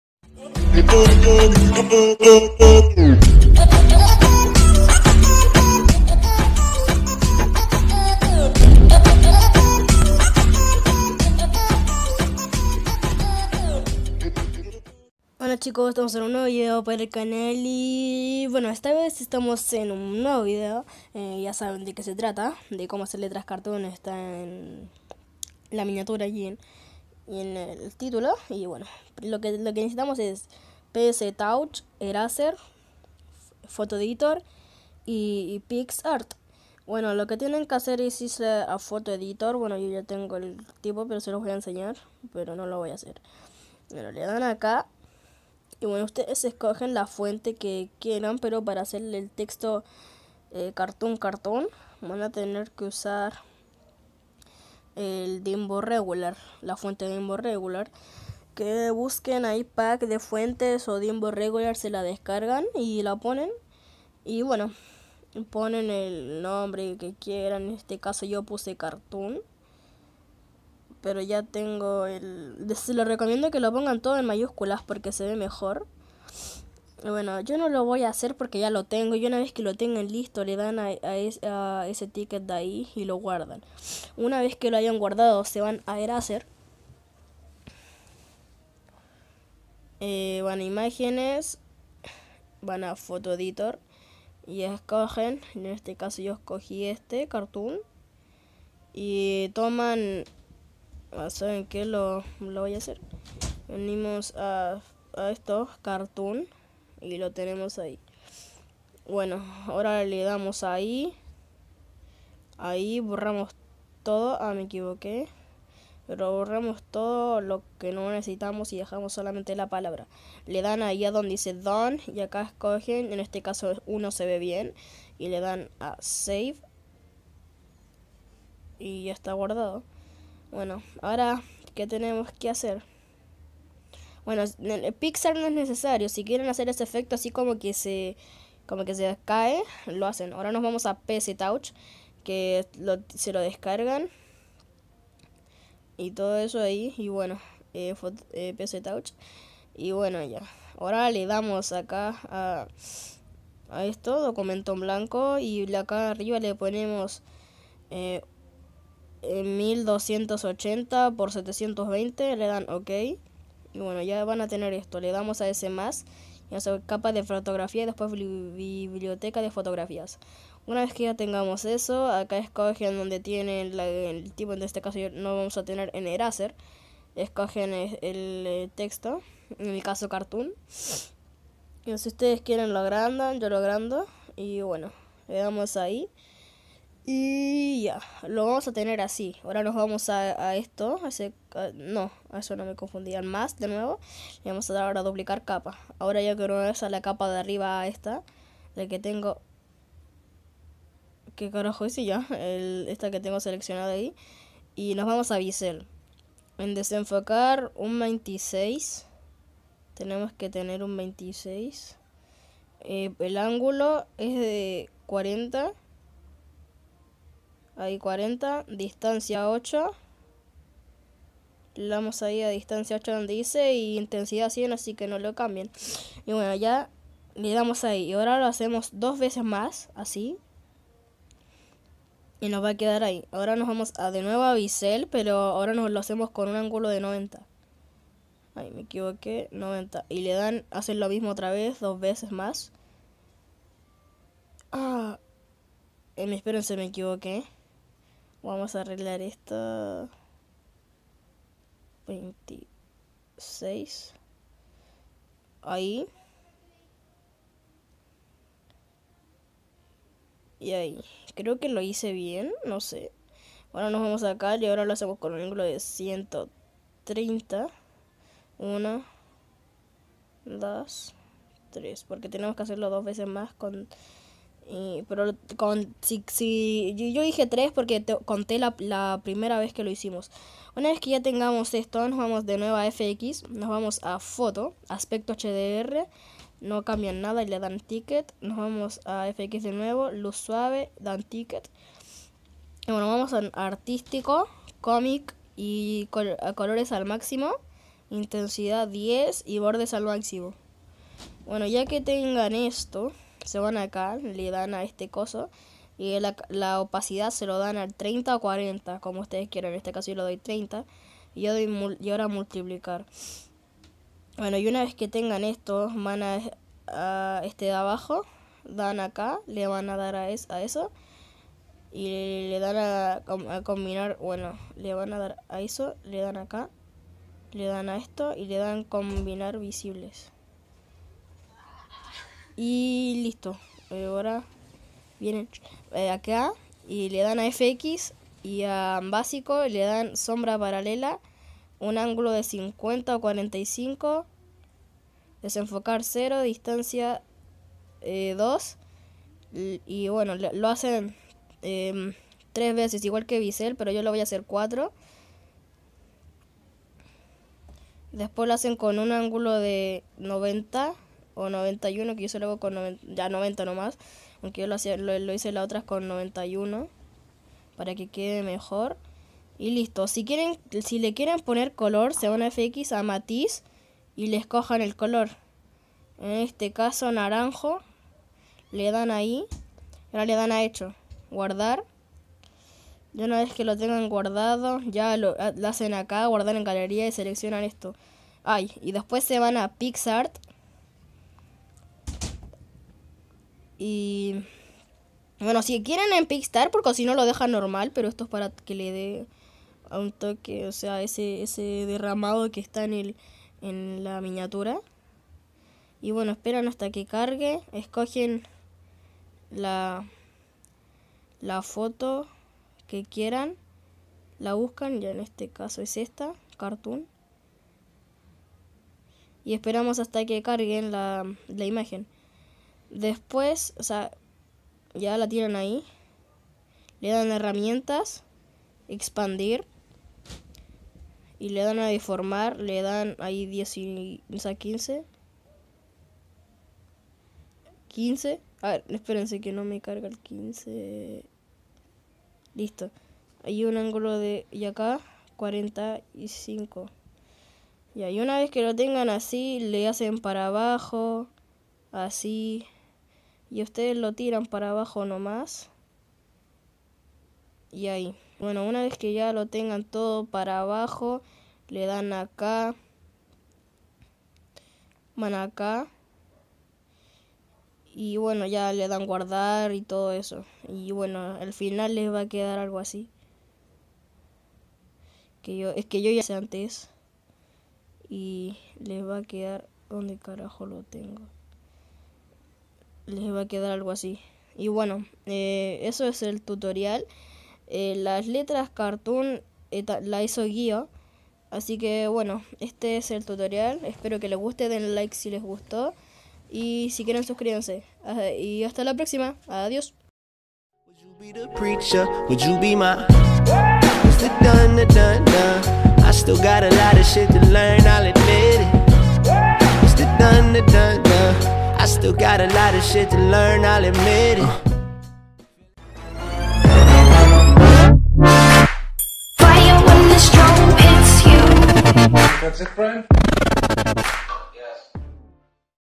Como hacer letras cartoon/ps touch
Hola chicos esta ves les traigo un tutora de como hacer letras cartoon espero que les guste y hasta la próxima bye